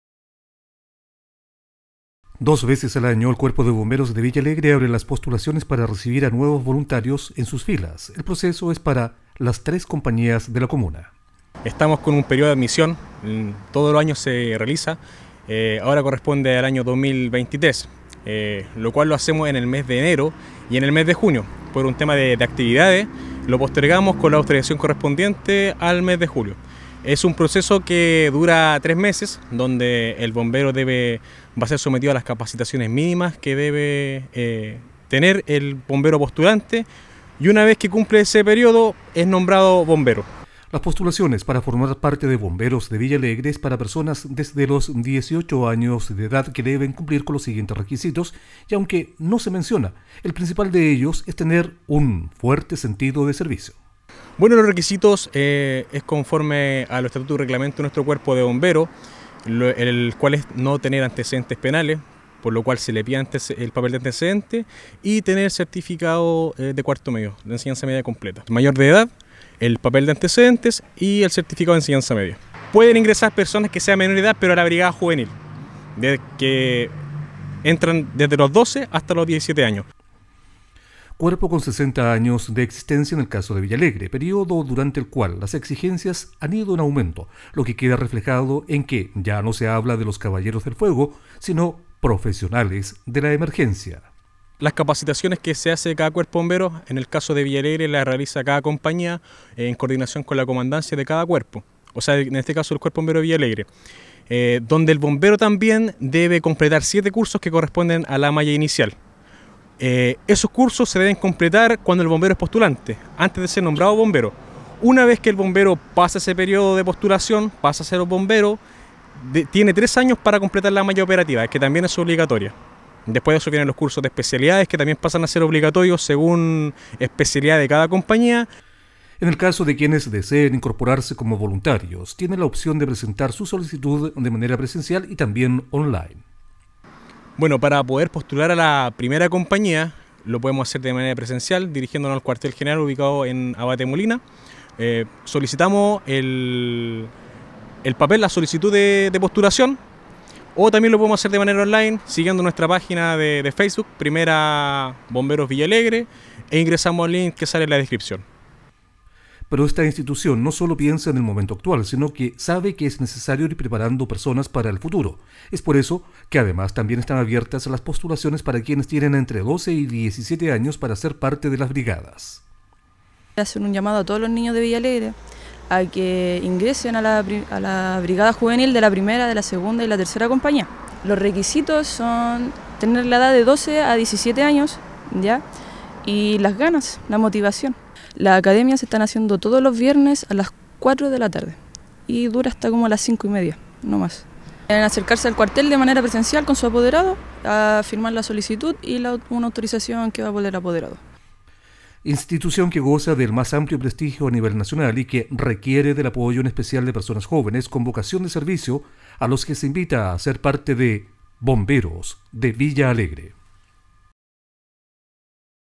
BOMBEROS DE VILLA ALEGRE ABRE POSTULACIONES
Dos veces al año, el Cuerpo de Bomberos de Villa Alegre, abre las postulaciones para formar parte de sus filas. Los requisitos para ser bombero voluntario entre otros son ser mayor de edad y tener 4º medio rendido.